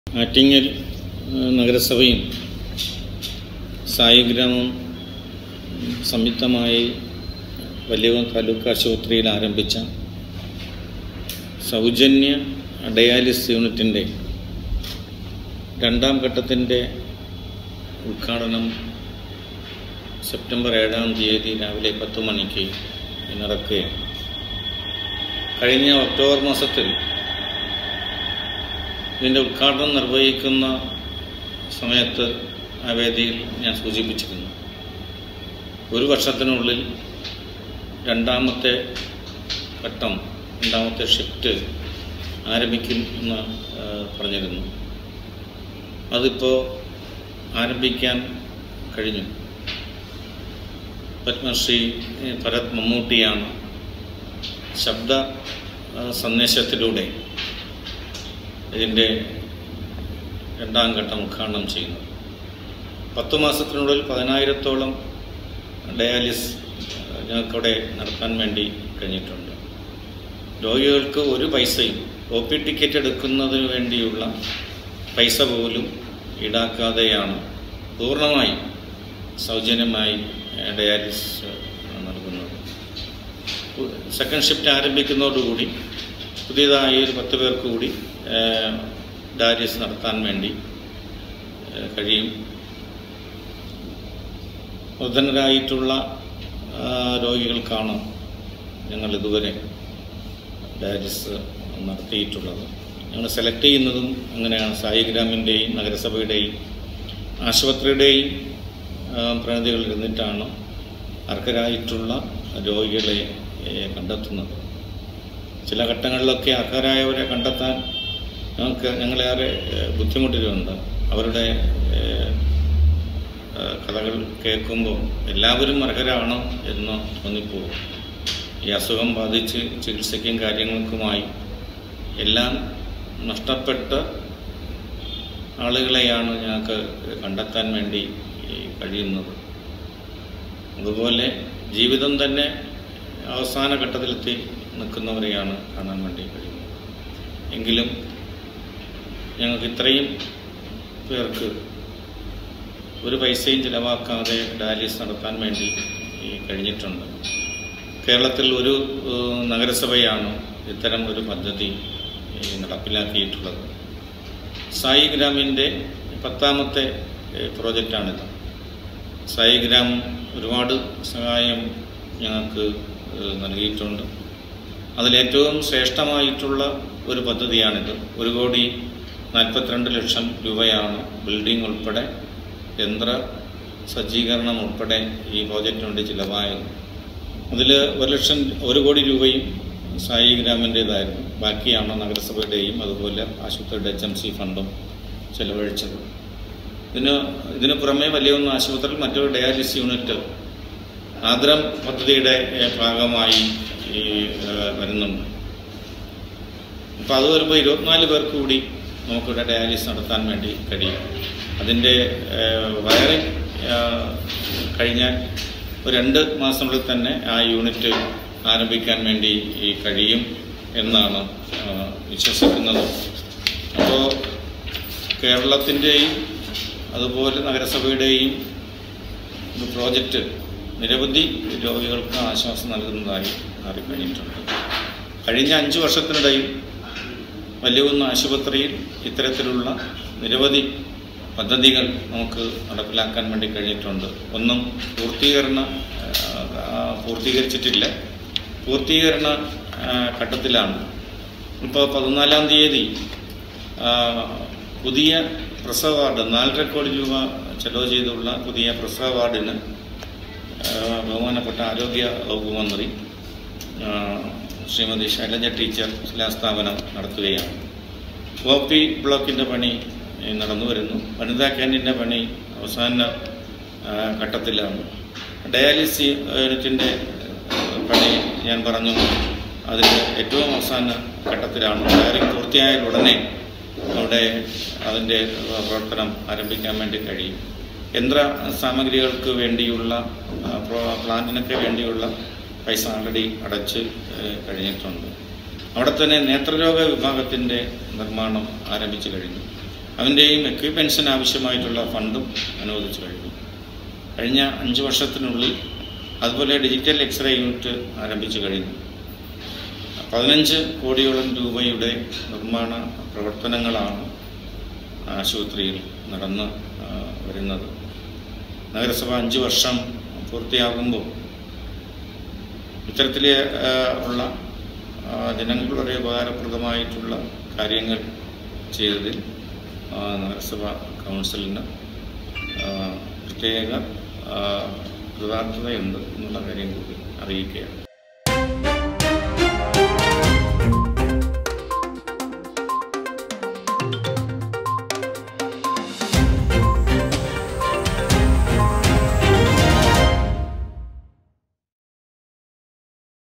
ഡയാലിസിസ് യൂണിറ്റിന്റെ രണ്ടാംഘട്ട ഉദ്ഘാടനം വലിയകുന്ന് താലൂക്കാശുപത്രിയിൽ മമ്മൂട്ടി നിർവ്വഹിക്കുന്നു
ആറ്റിങ്ങൽ നഗരസഭയുടെയും സായിഗ്രാമിന്റെയും സംയുക്ത സംരംഭമായ സൗജന്യ ഡയാലിസിസ് യൂണിറ്റിന്റെ രണ്ടാം ഘട്ട ഉദ്ഘാടനം വലിയകുന്ന് താലൂക്കാശുപത്രിയിൽ സെപ്റ്റംബർ ഏഴിന് പത്മശ്രീ ഡോ. ഭരത് മമ്മൂട്ടി നിർവ്വഹിക്കുന്നു